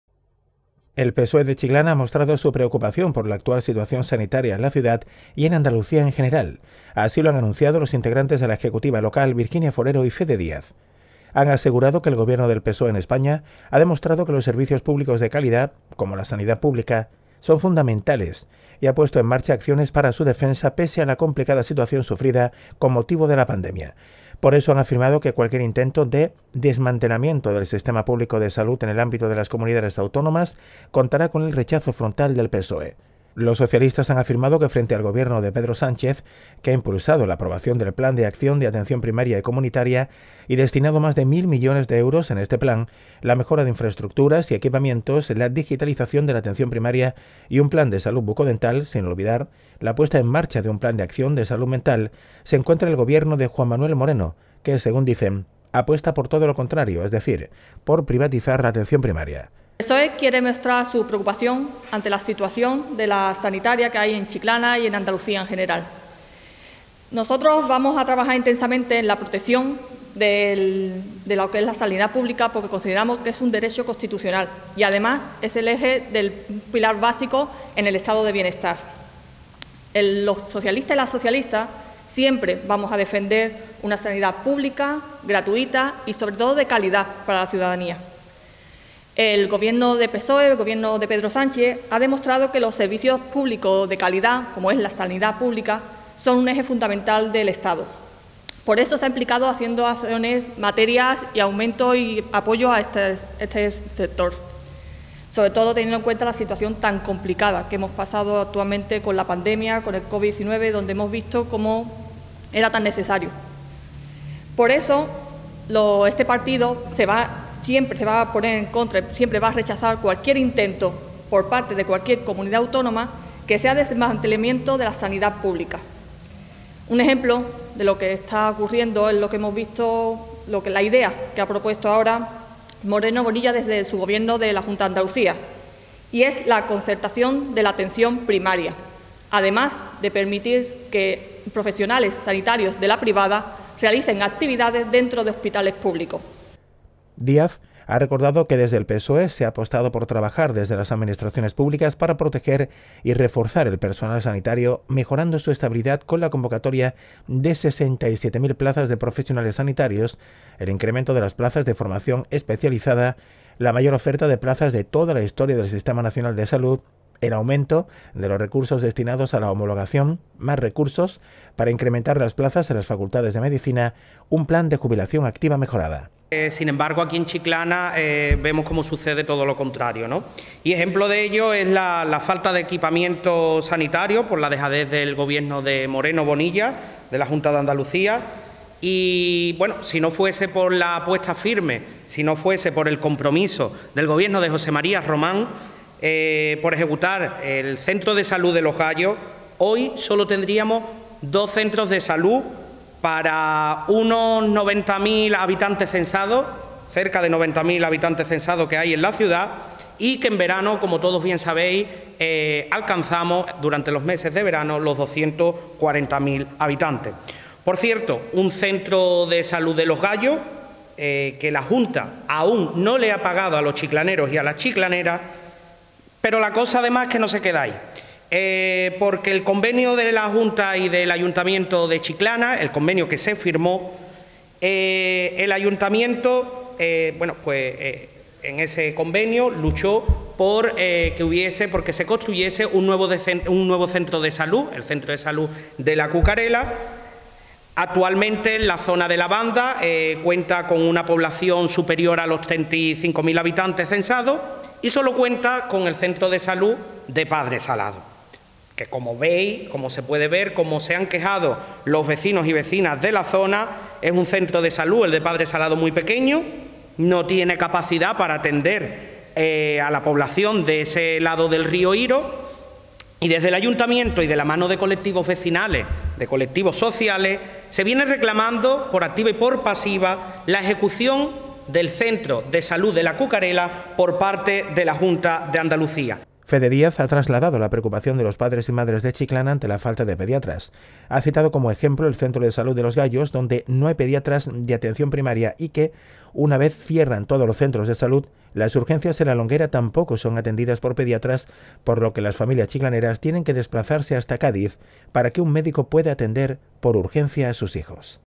EL PSOE DE CHICLANA MUESTRA SU PREOCUPACIÓN POR LA ACTUAL SITUACIÓN SANITARIA EN ANDALUCÍA
El PSOE muestra su preocupación por la actual situación sanitaria en la ciudad y en Andalucía. Fede Díaz recuerda que “al PP tampoco le preocupa la situación que están sufriendo los padres y madres ante la falta de pediatras en nuestra ciudad. Y ejemplo de ello es que en el centro de salud de Los Gallos directamente no hay pediatras de atención primaria”